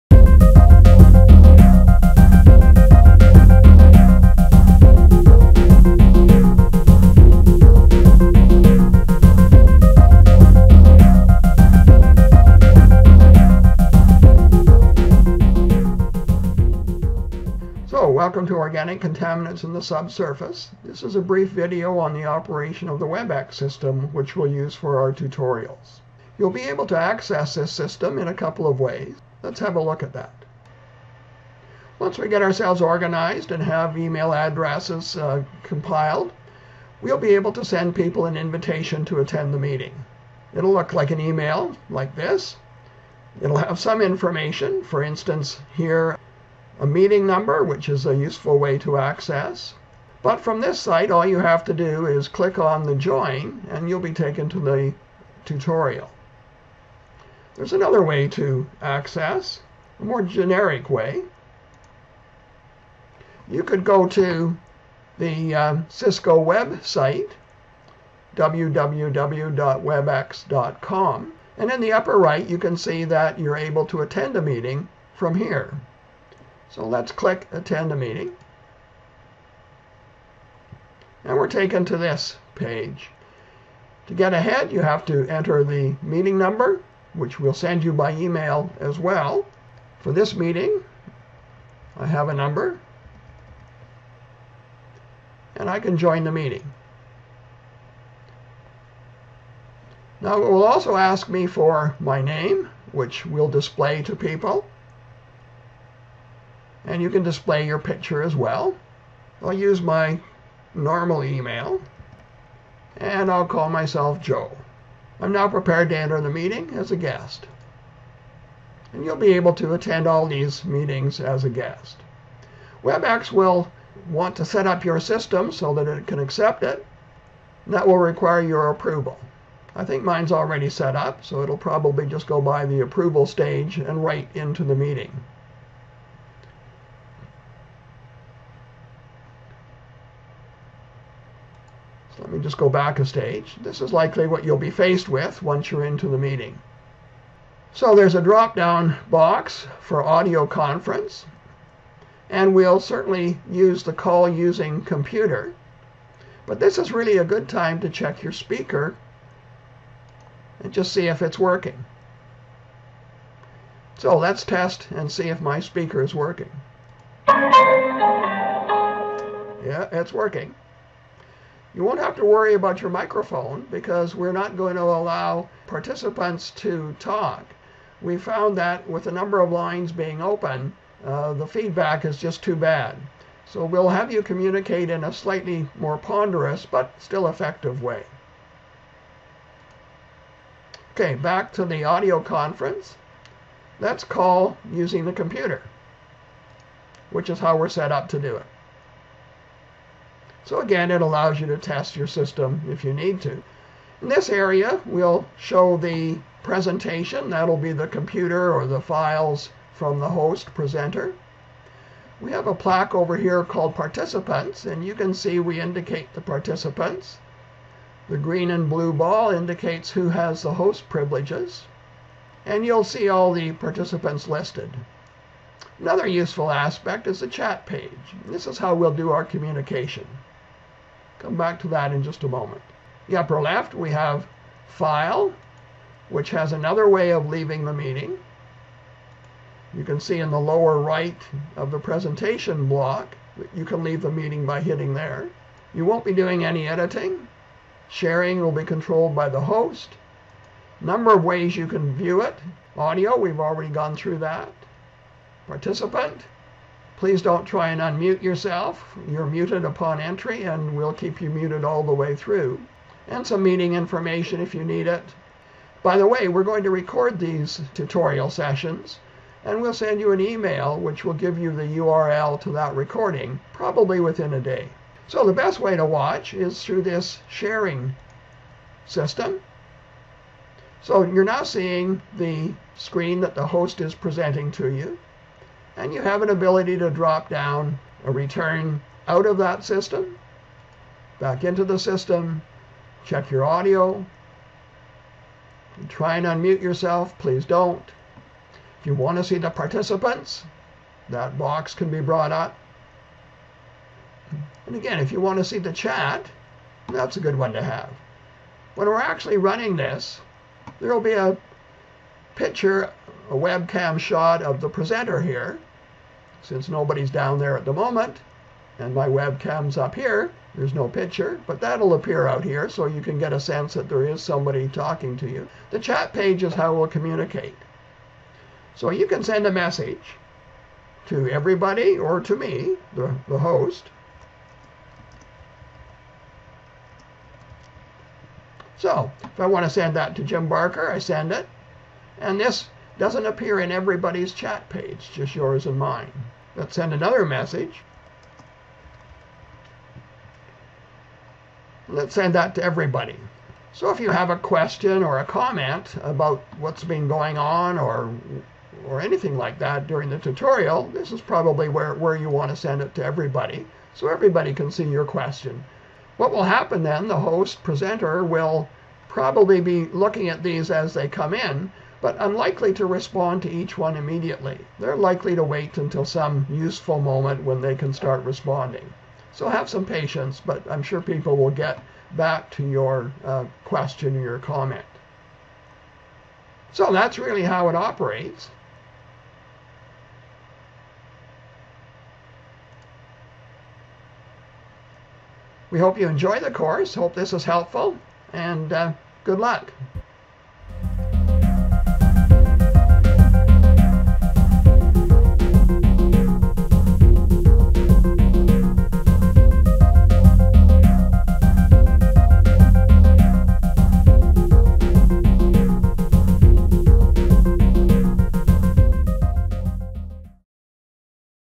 Organic Contaminants in Subsurface Environment XVfdrKrR
Professors: Jim Barker (University of Waterloo, Canada), Everton de Oliveira (Hidroplan and UNESP Rio Claro, Brazil), Jason Gerhard (University of Western Ontario, Canada), Massimo Marchesi (Politecnico di Milano, Italy), John Molson (Université Laval, Canada), Marco Petitta (La Sapienza, Italy) and Neil Thomson (University of Waterloo, Canada)